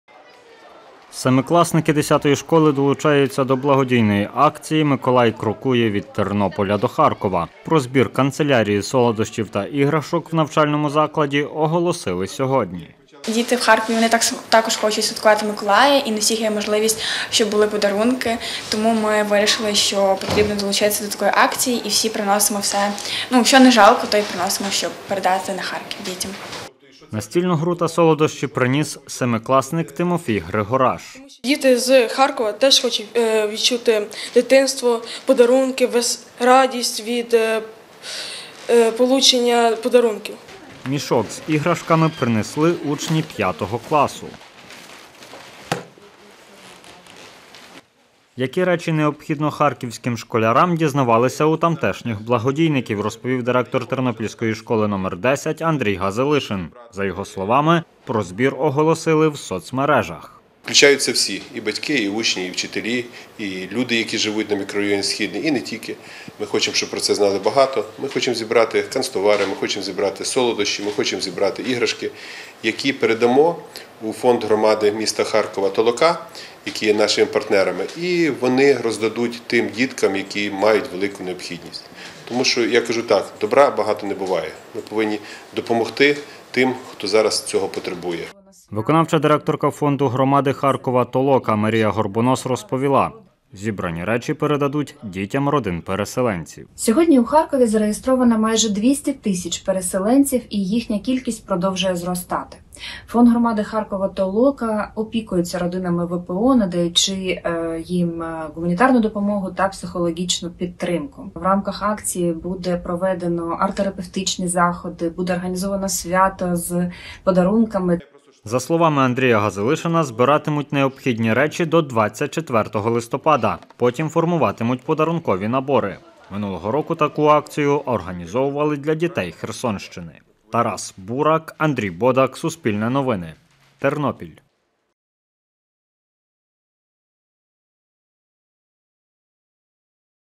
«Миколай крокує від Тернополя до Харкова»: у школі стартувала акція
Благодійну акцію «Миколай крокує від Тернополя до Харкова» організували в десятій школі міста. Під час неї учні та вчителі закладу збирають канцтовари, іграшки та солодощі для школярів з Харкова, розповів директор школи Андрій Газилишин. За його словами, до Дня святого Миколая таку акцію в школі організовують вдруге. Минулого року проводили для учнів Херсонщини.